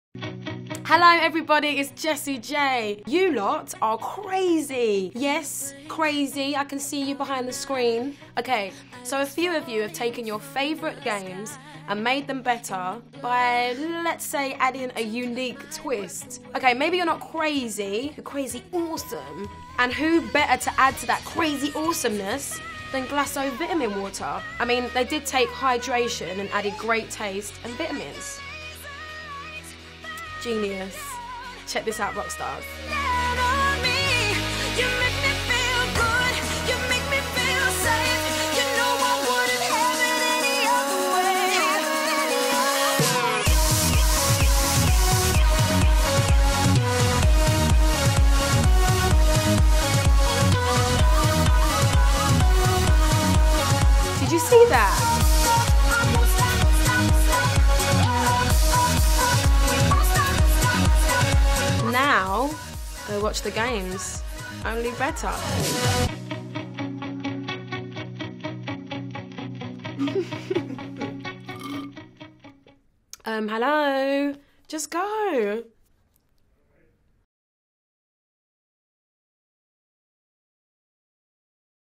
glaceau vitaminwater plays games, only better with Jessie J
you and vitaminwater have taken the games we grew up with and reinvented them with totally new, suprising twists.think of these as the glaceau vitaminwater of games. putting a twist on hydration. putting a twist on games. yup, we love making games more fun, and as it turns out -- -- so do you.